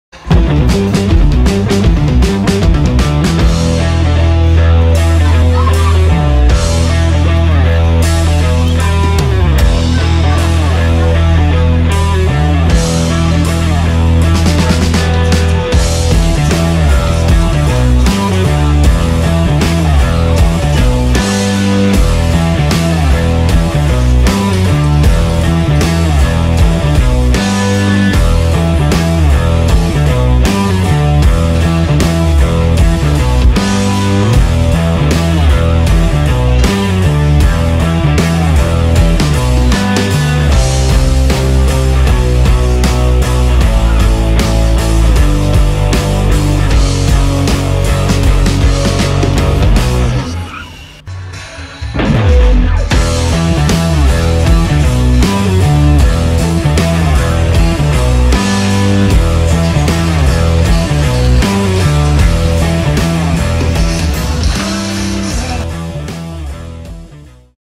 Extreme Shenanigans!
Non-stop rock 'n roll shenanigans at the Flying Panda Extreme Airsports Park!